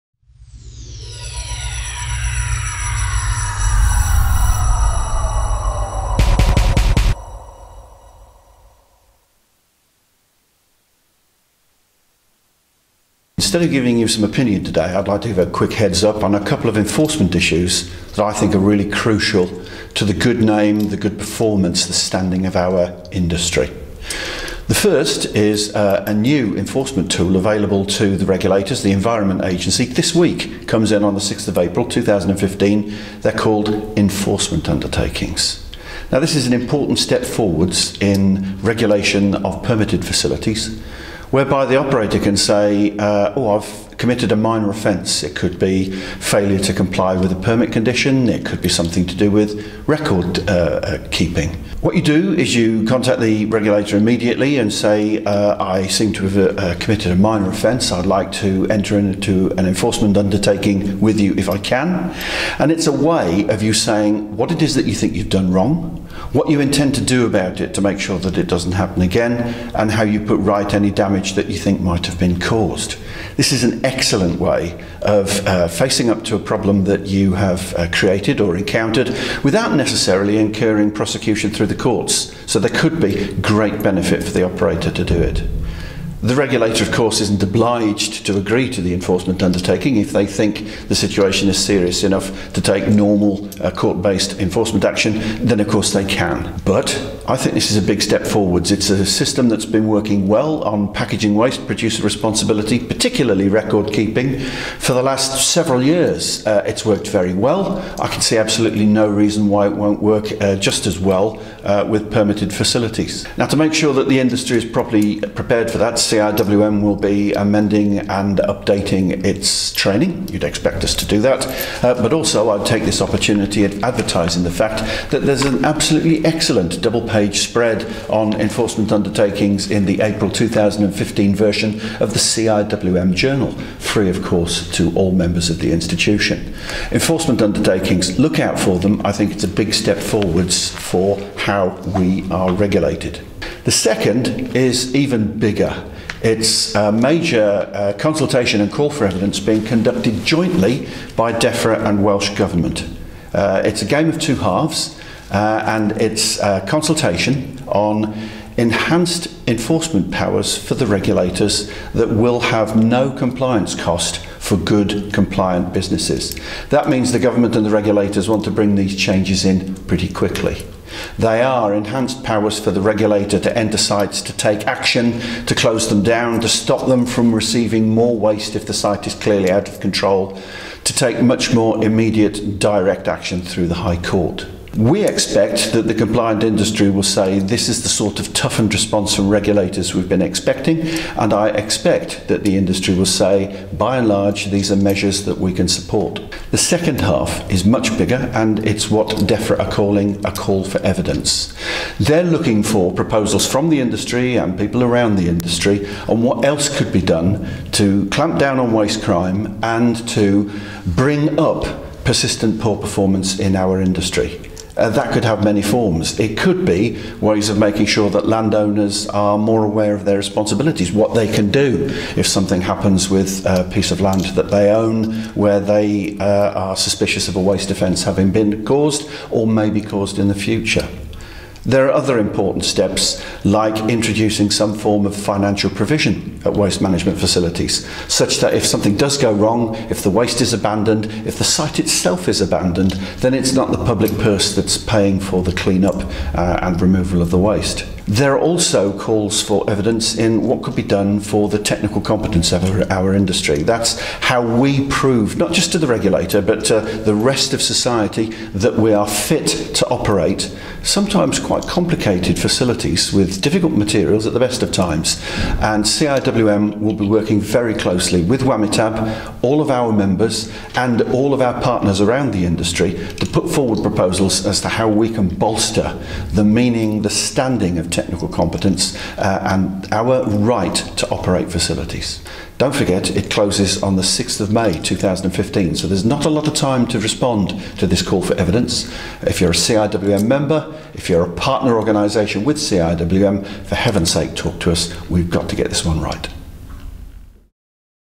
CIWM's Steve Lee on Enforcement Undertakings
CIWM's chief executive, Steve Lee, discusses Enforcement Undertakings, enhanced enforcement powers for regulators and what else can be done to clamp down on waste crime